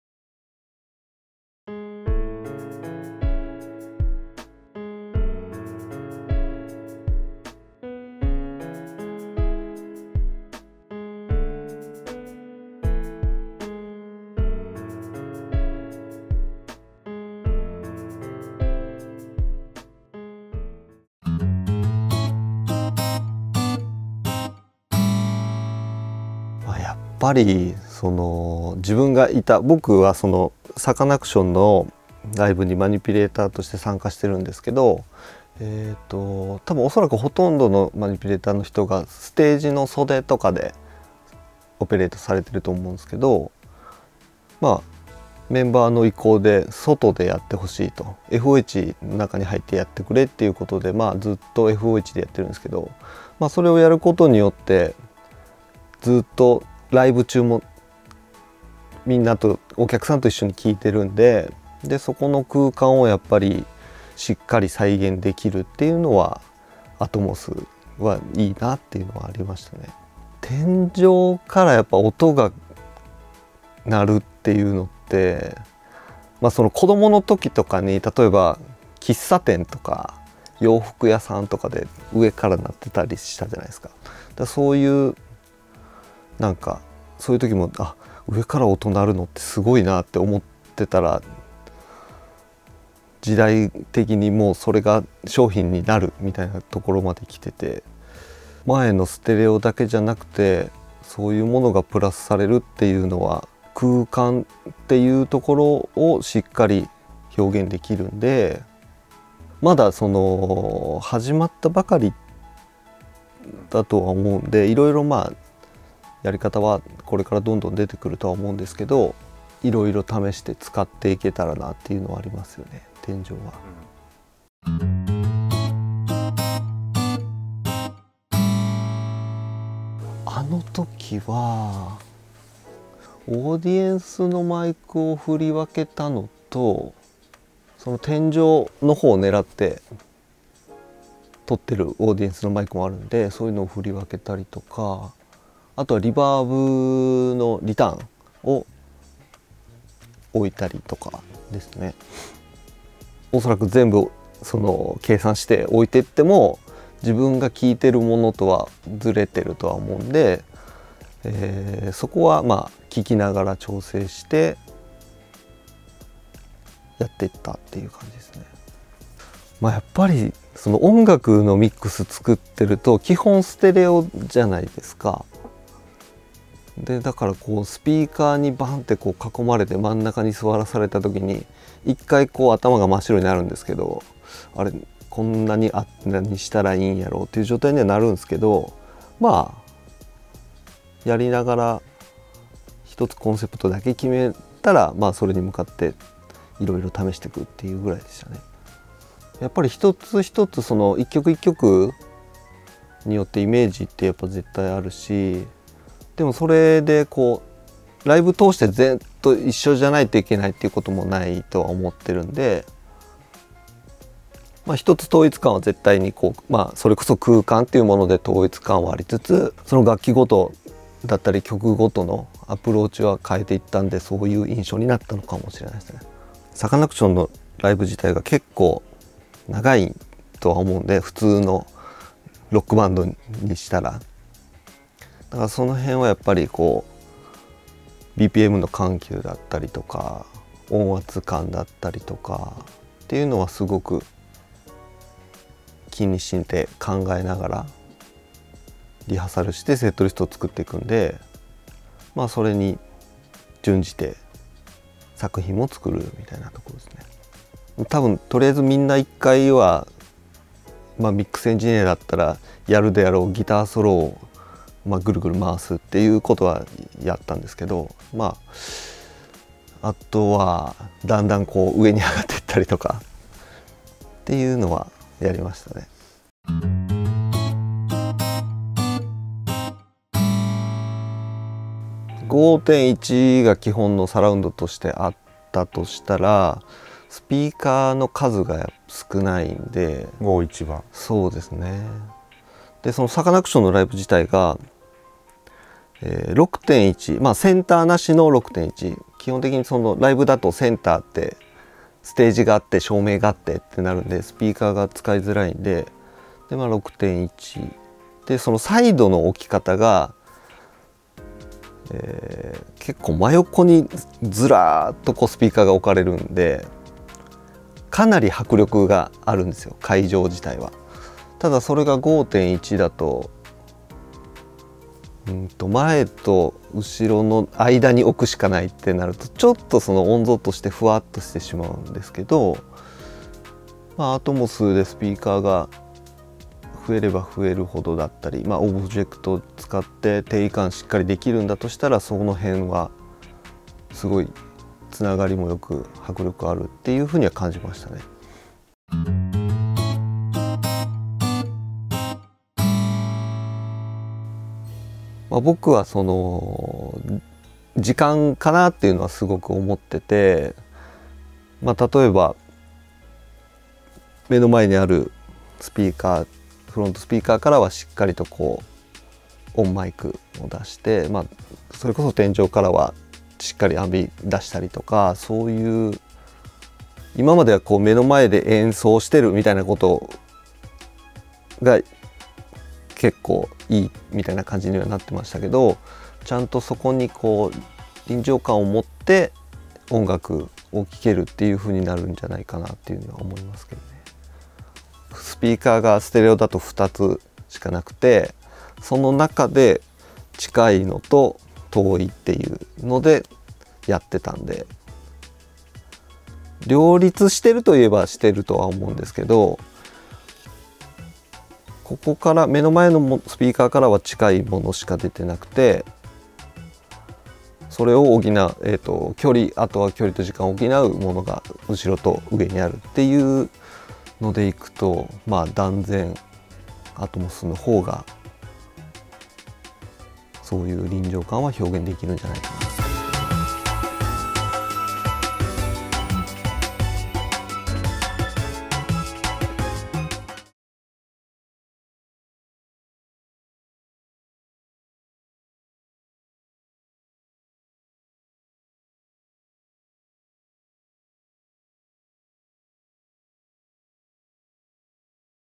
Masashi Uramoto INTERVIEW – Part 1 | Dolby Atmos Music Creator’s Summit
Aobadai Studio チーフ・エンジニア/サウンド・プロデューサーでサカナクション、Eve、KID FRESINO等の作品を手がけてきた浦本雅史氏が、ドルビーアトモス を使った楽曲制作、ミックスの魅力、可能性、コツについて語ります。